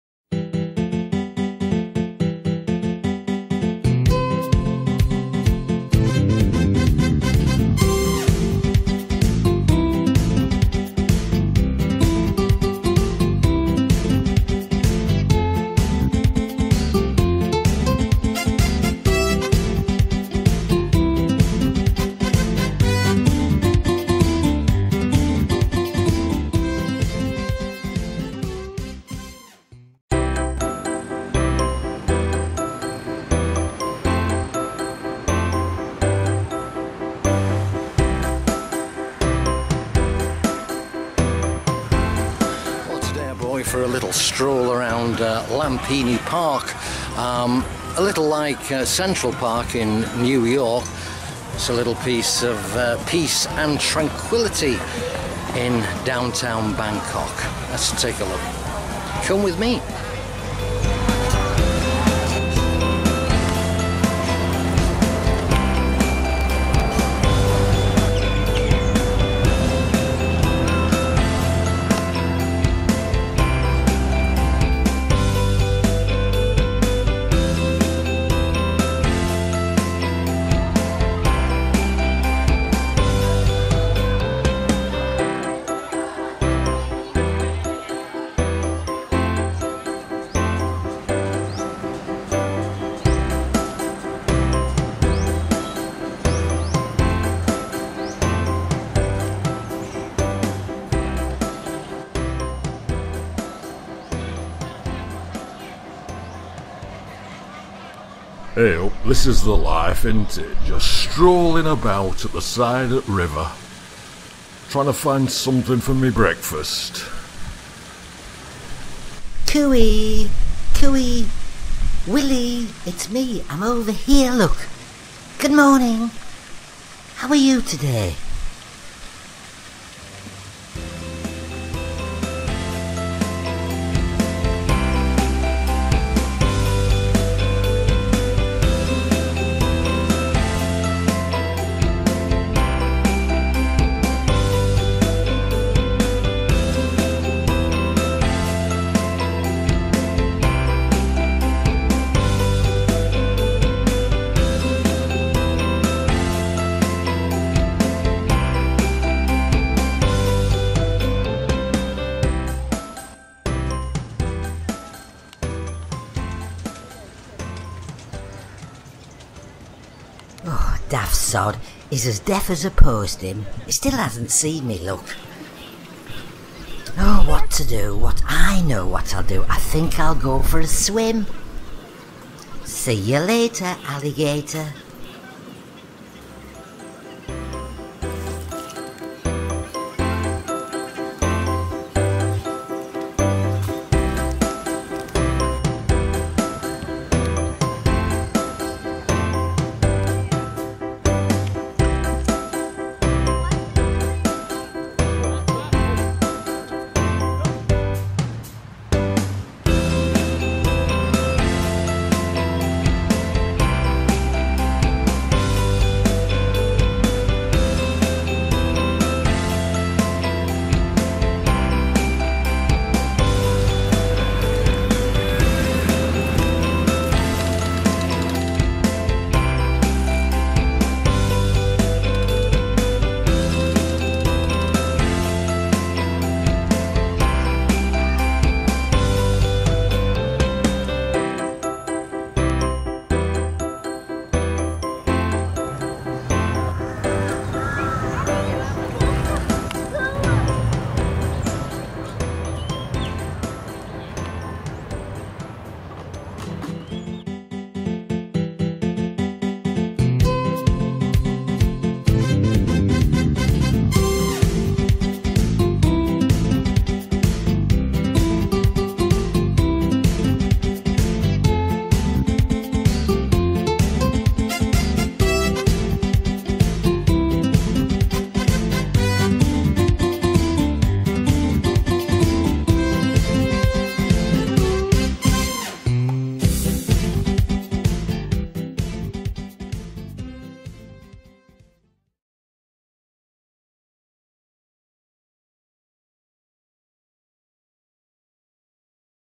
Lumphini Park Episode 7
Peter and the Grimsdales Do Bangkok - Episode 7 (Lumphini Park). In this episode, there`s a surprise in store as some of the Grimsdales can be seen as well heard! If you`re old enough to remember Johnny Morris (BBC Animal Magic) then you`ll enjoy this video filmed in Bangkok`s  Lumphini Park with the Monitor Lizards.